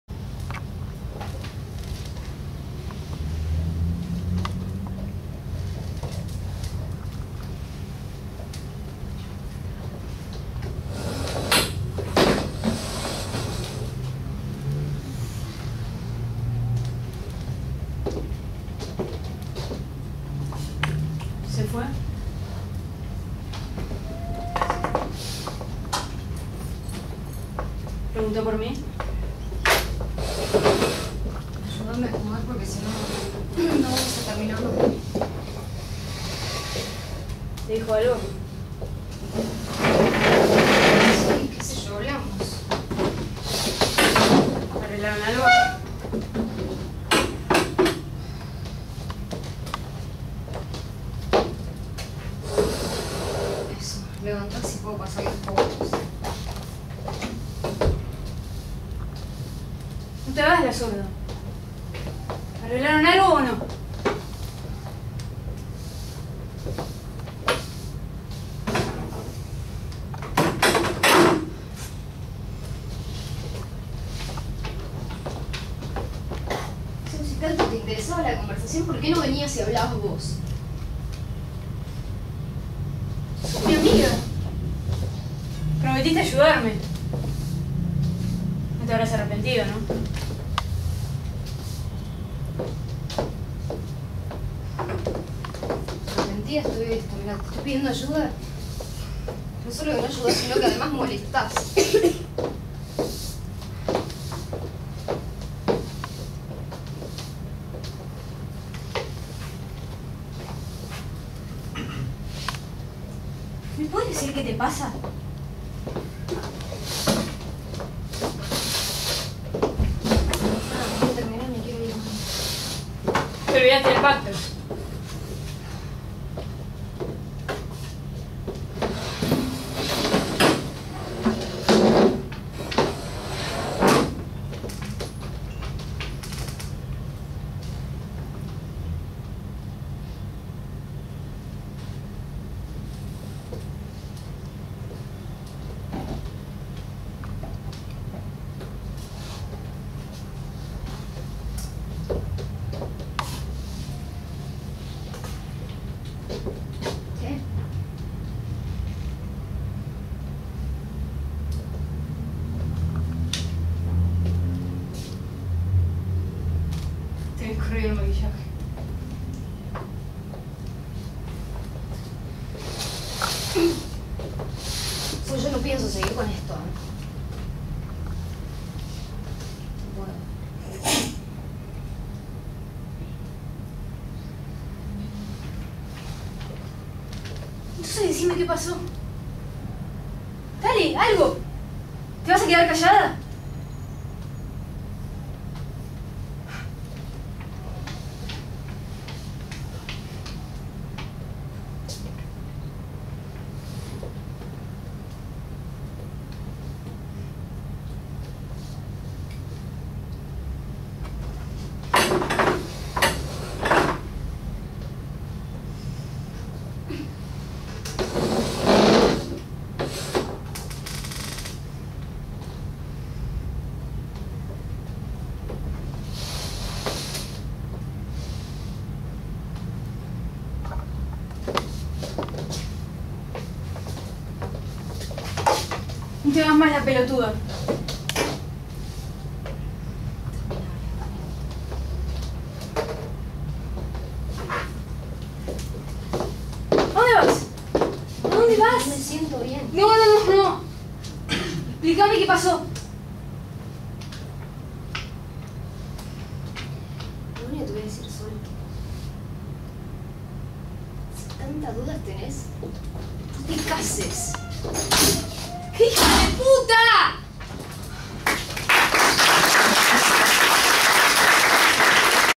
Bar. Sol De Fazio y Caro
Muestra Primer Año Actuación. Prof: Debora Astrosky. Tercer Acto, Escuela de Teatro 2013.